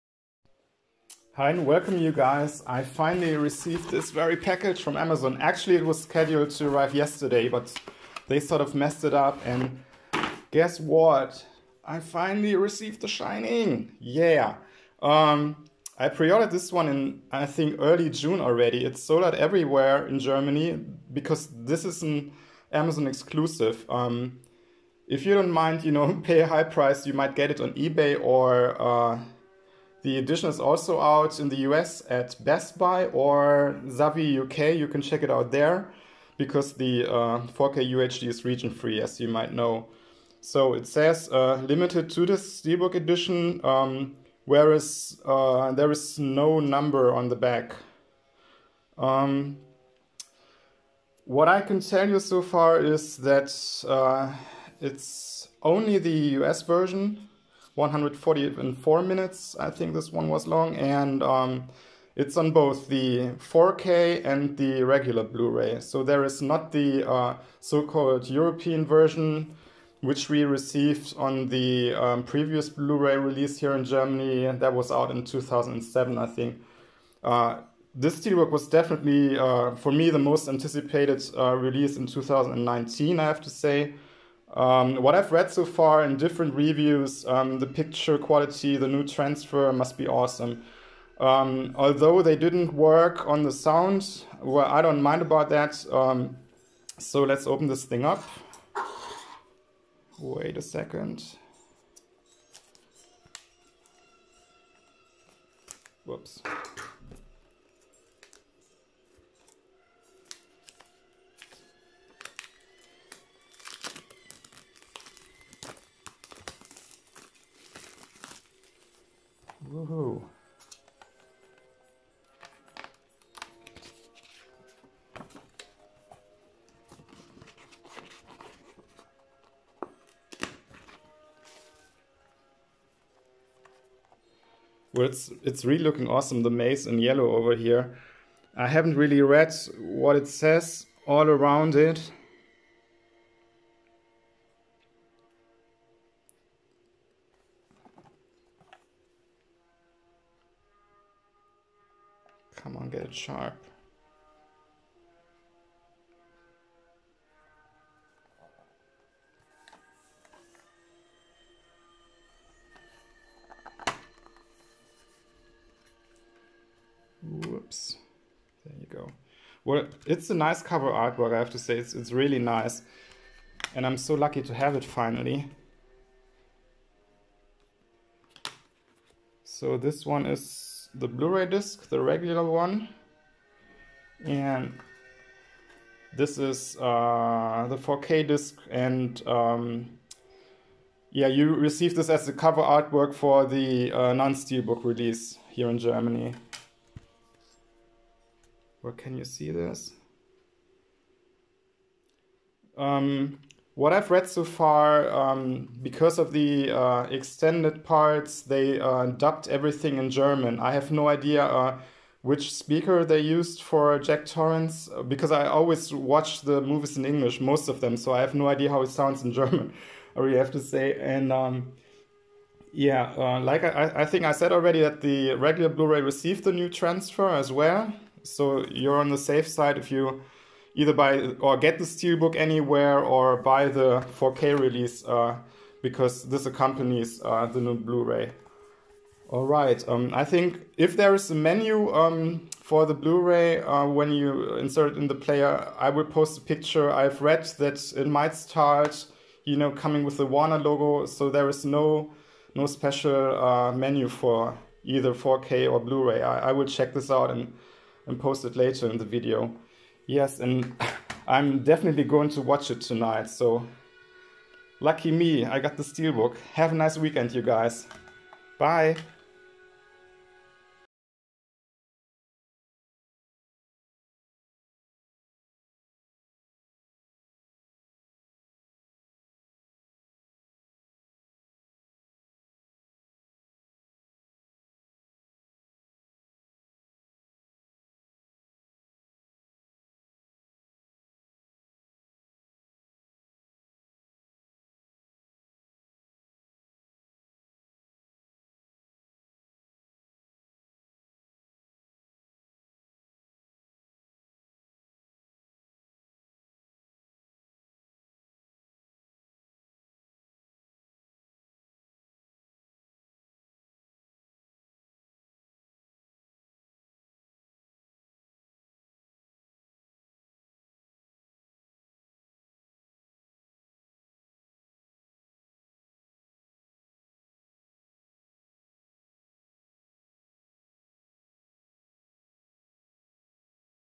⛏️❄️ The Shining 4K UHD Limited Steelbook Edition - Amazon Germany Exclusive 11.10.2019
I finally received my copy today.
The steelbook is an Amazon Exclusive here in Germany.

You only got the US cut on both, the 4K UHD and Blu-ray disc.

Bonus:
Audio Commentary - featuring Steadicam inventor Garrett Brown and Kubrick biographer John Baxter
View from the Overlook: Crafting "The Shining" (30:22)
The Visions of Stanley Kubrick (17:17)
The Making of "The Shining" (34:59) - with optional audio commentary by Vivian Kubrick
Wendy Carlos, Composer (7:31)